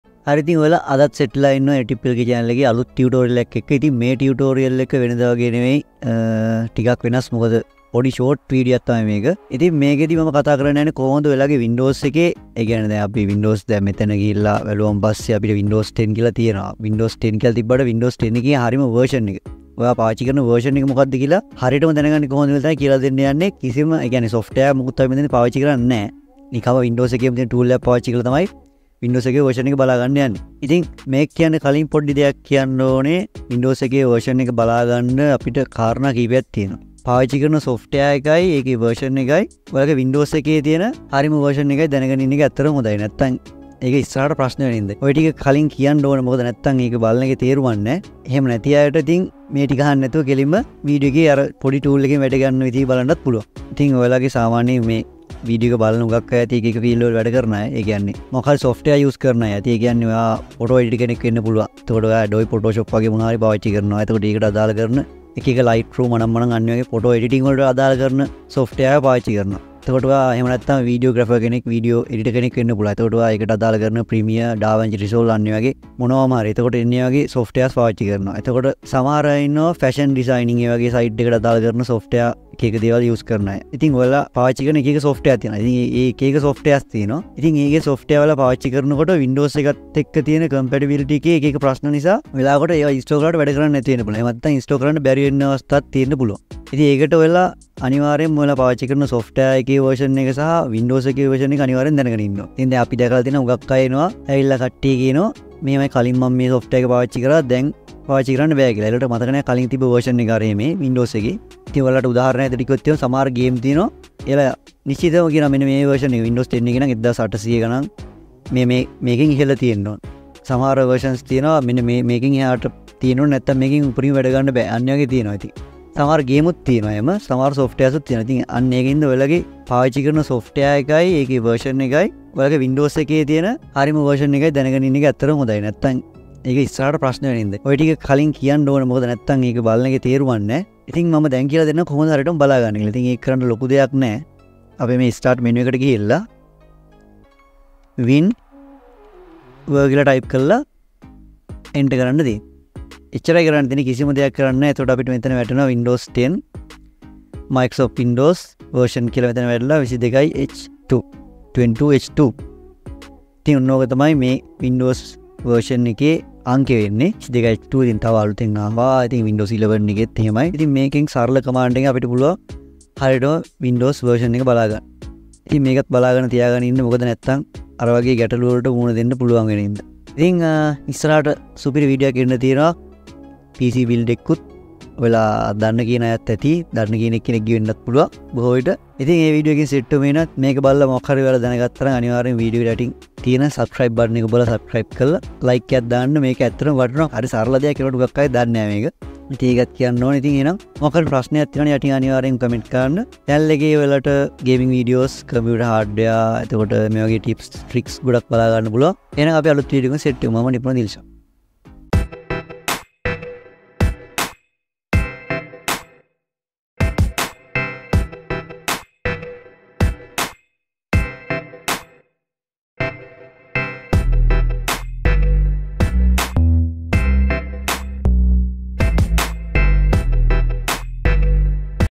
How to check windows 10 version | Sinhala | Anytiplk
How to check Windows 10 version

⭕My friends today i broght a very short sinhala tutorial.so in this videos we are going to discuss how check windows 10 version.did you know there are so many benefits we can get with knowing windows 10 version.i will explain all about in this video.
then let's see how to check the Windows 10 version.

                                                                           Thanks, Anytiplk.

⭕මගේ යාළුවනේ අද මම ගෙනාවේ ඉතා කෙටි සිංහල නිබන්ධනයක්.ඉතින් මේ වීඩියෝ එකෙන් අපි සාකච්ඡා කරන්න යන්නේ windows 10 version එක චෙක් කරන්නේ කොහොමද කියලා.ඔයාල දන්නවද windows 10 version එක දැනගැනීමෙන් අපිට ලැබෙන ප්‍රයෝජන ගොඩක් තියෙනවා කියලා.මම මේ වීඩියෝ එකේ ඔක්කොම විස්තර කරන්නවා .
එහෙනම් අපි බලමු කොහොමද Windows 10 version එක බලන්නේ කියලා.

                                                                             සැමට ස්තුතියි.